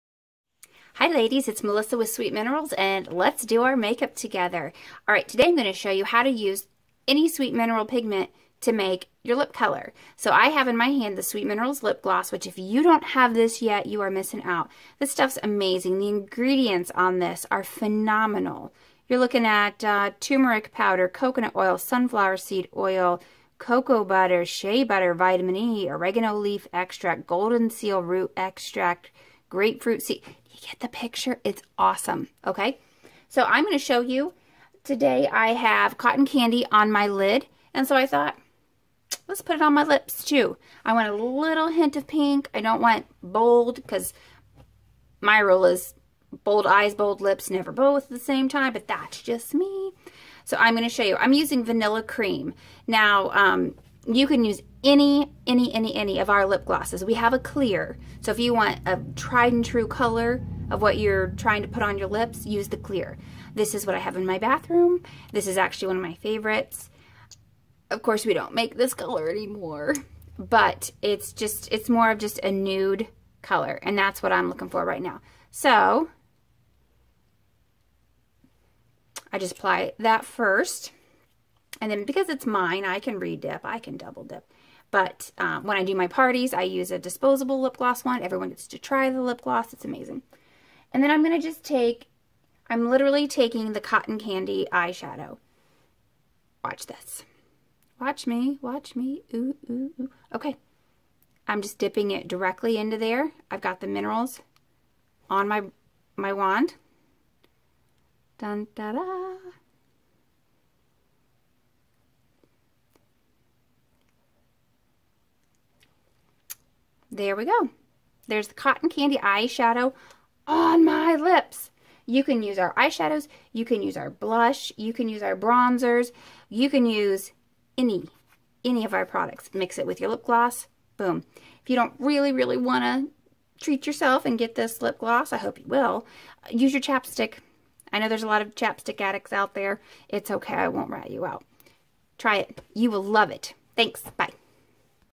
Sweet Minerals - Lipgloss mixed with eye color!?
Did you know you can use ANY Sweet Minerals pigments anywhere? Blush on eyes, bronzer on lips, eyeshadow on lips... Blush on lips...you get the picture?! Snag our naturally based lip gloss - you'll never wear another brand again!